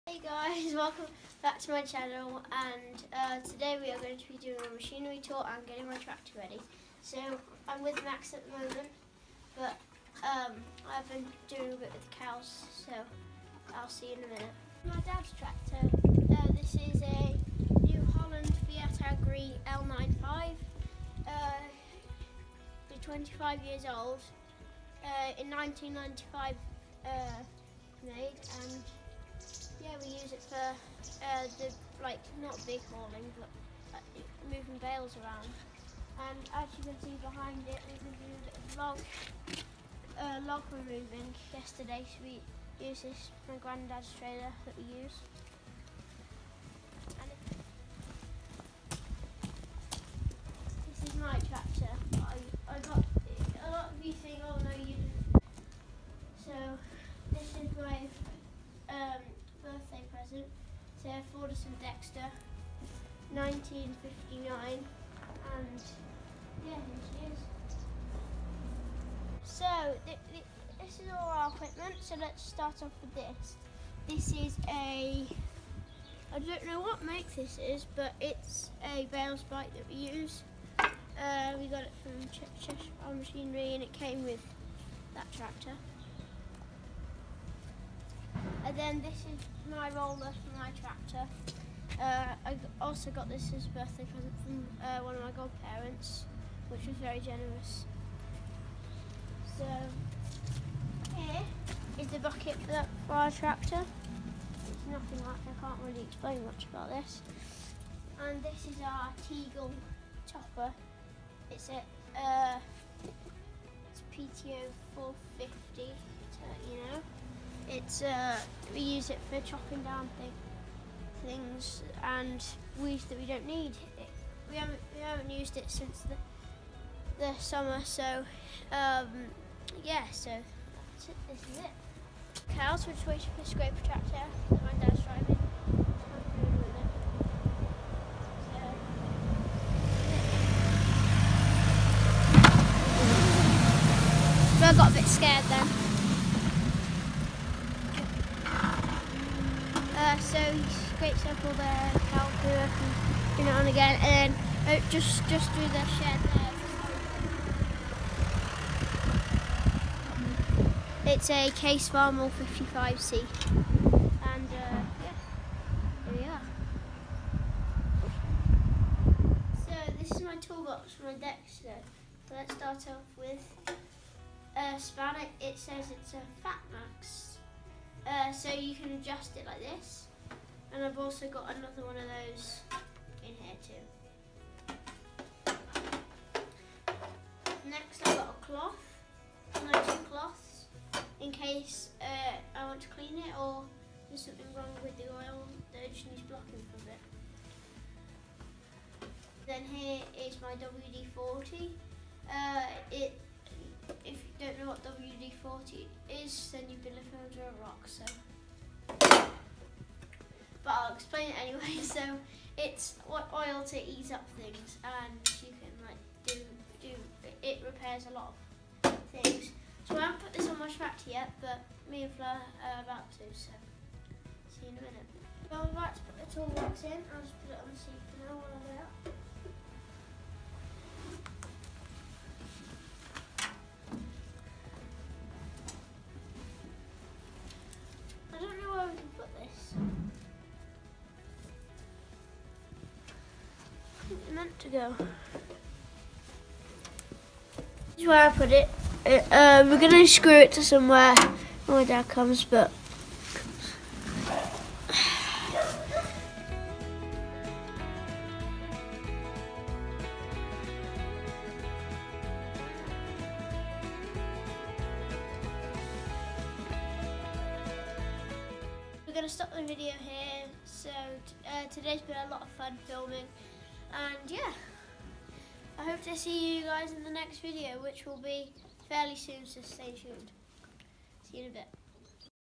Machinery Tour! (Preparing my tractor) 🚜
Hey guys,today I filmed a machinery tour where I show you all my equipment, I also attached some things to my tractor! 🚜